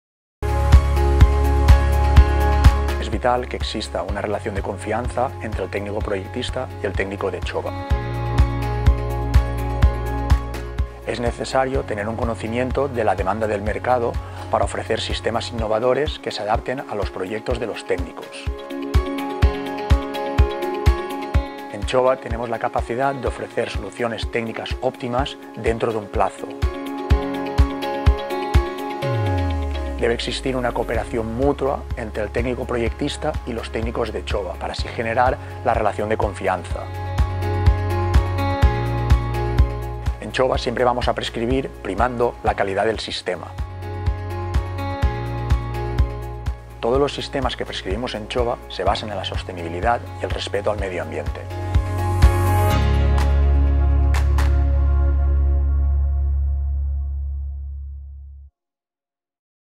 ChovA - Las 6 C que garantizan la prescripción
Juan Ripoll nos desvela las seis C de ChovA que garantizan una prescripción responsable.